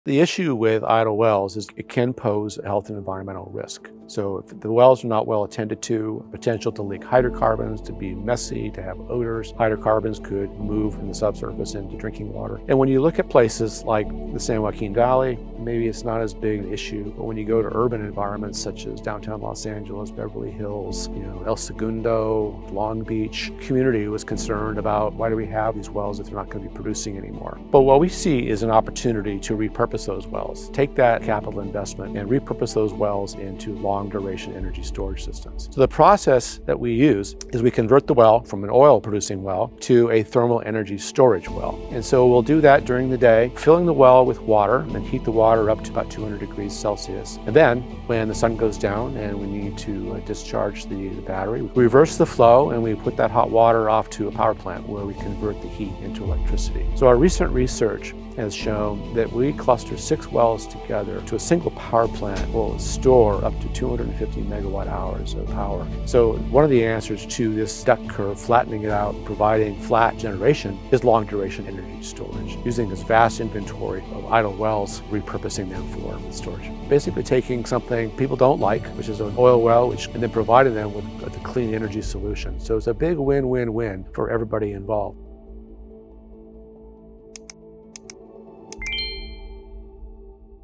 Why We Use Idle Oil Wells to Store Energy
California has more than 39,000 idle oil wells, many of which pose environmental and financial risks if left unaddressed. While often treated as a burden, former state energy regulator Bill Bartling sees a missed opportunity. In this episode, he shares how a tech-driven solution could cut emissions and help stabilize the grid, but only if the state is willing to act.

Views expressed in this video are the opinions of the host and guests and do not necessarily reflect the views of California Insider.